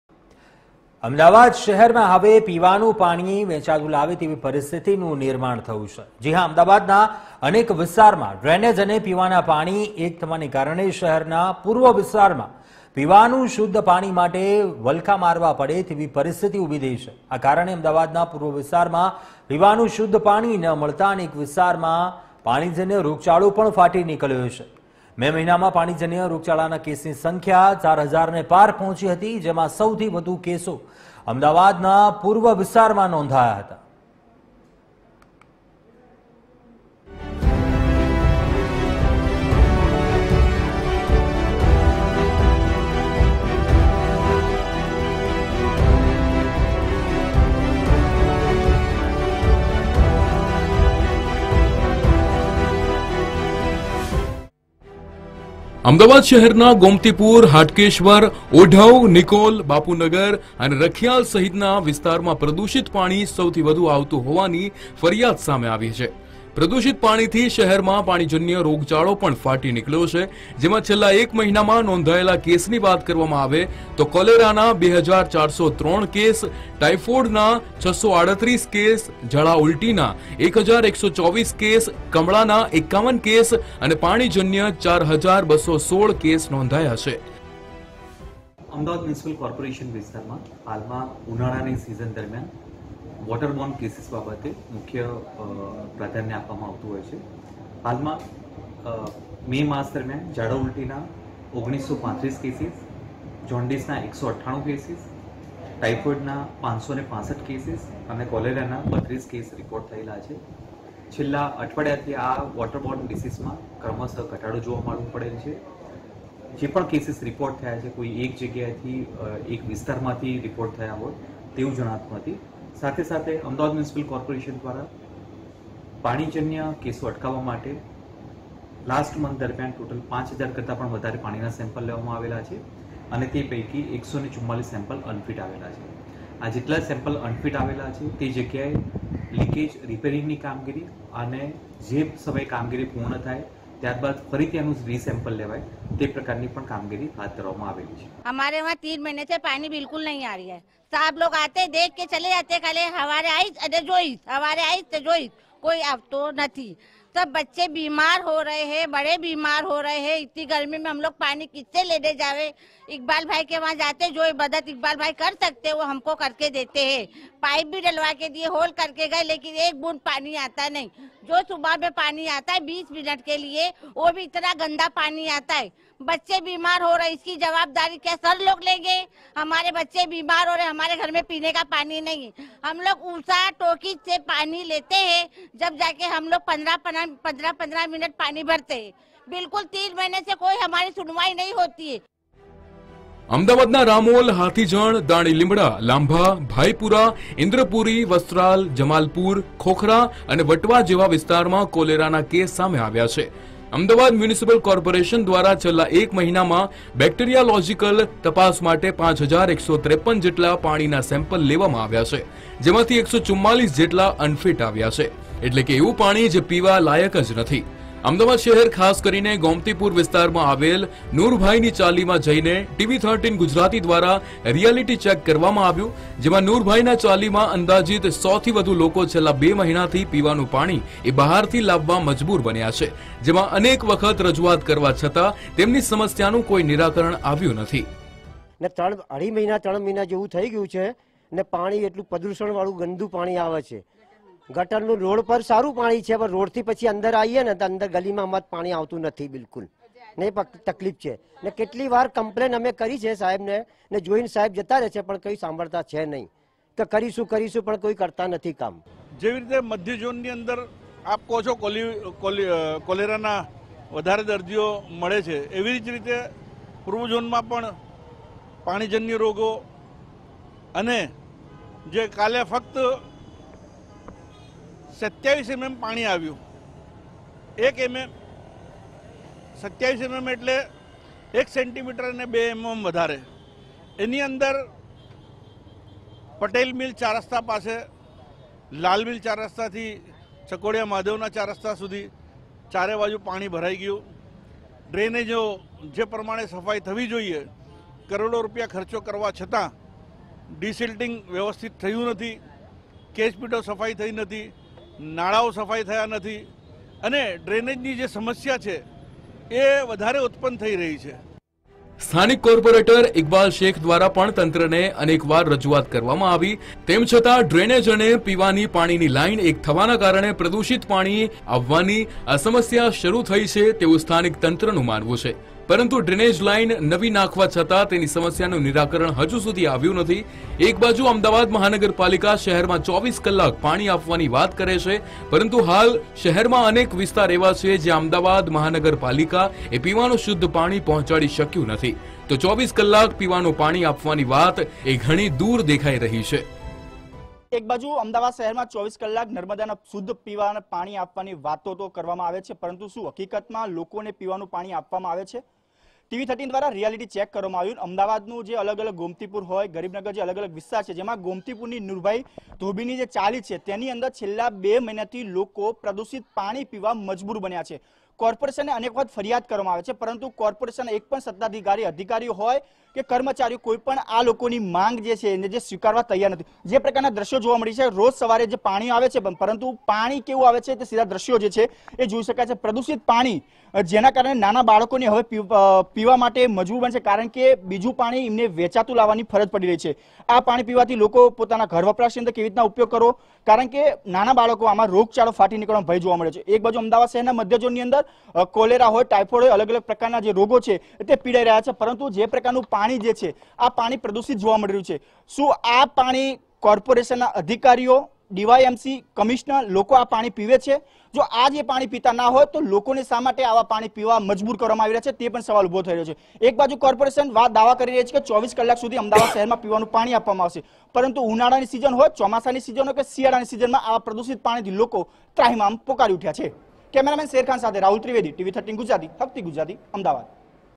Ahmedabad :પાણીજન્ય રોગચાળો ફાટી નિકળ્યો
અમદાવાદના અનેક વિસ્તારમાં ડ્રેનેજ અને પીવાના પાણી એક થવાના કારણે શહેરના પૂર્વ વિસ્તારમાં પીવાનુ શુધ્ધ પાણી માટે વલખા મારવામાં આવે તેવી પરિસ્થિતી ઉભી થઇ છે. જેના કારણે અમદાવાદના પૂર્વ વિસ્તારમાં પીવાનું શુધ્ધ પાણી ના મળતા અનેક વિસ્તારમાં પાણી જન્યરોગચાળો ફાટી નિકળ્યો છે.